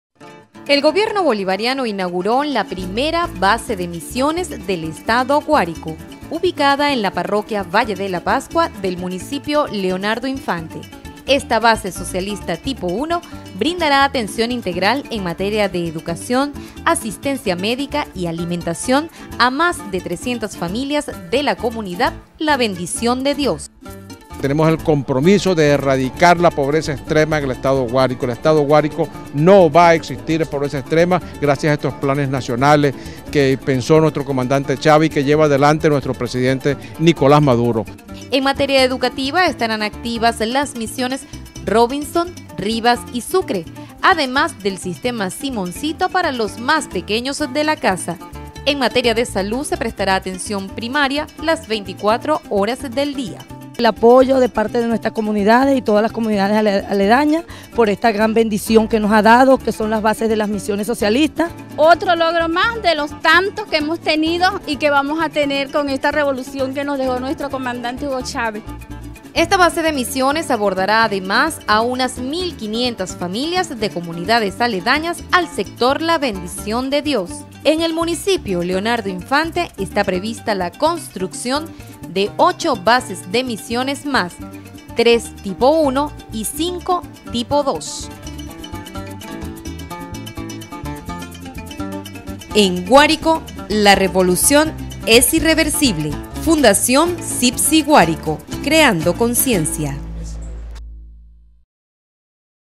BASES DE MISIONES SOCIALES: "BENDICION DE DIOS"
La Base de Misiones se ubica en el municipio Leonardo Infante en la ciudad de Valle de la Pascua, sector Las Acacias, con servicios de educación, medicina integral y alimentación, es la primera de 82 bases de misiones sociales que serán instaladas en todo el territorio guariqueño